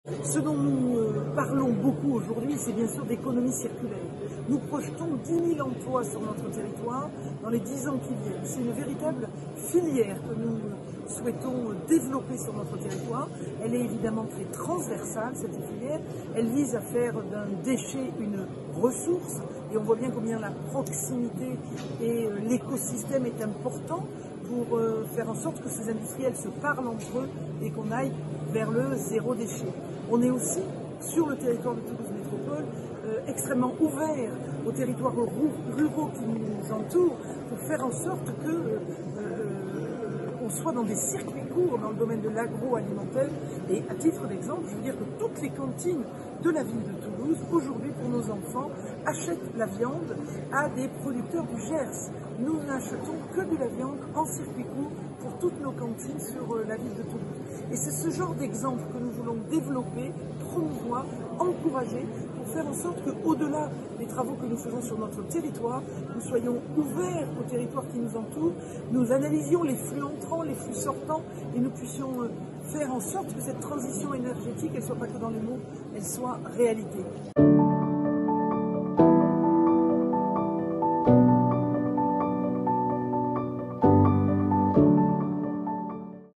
#1 Dominique Faure - L'économie circulaire, un modèle économique adopté par Toulouse Métropole ?
Rencontre Renaissance Industrielle du 19 octobre 2021 en région Occitanie autour du thème "Industrie et Environnement".

Interview de Dominique Faure, 1ère Vice-Présidente Toulouse Métropole.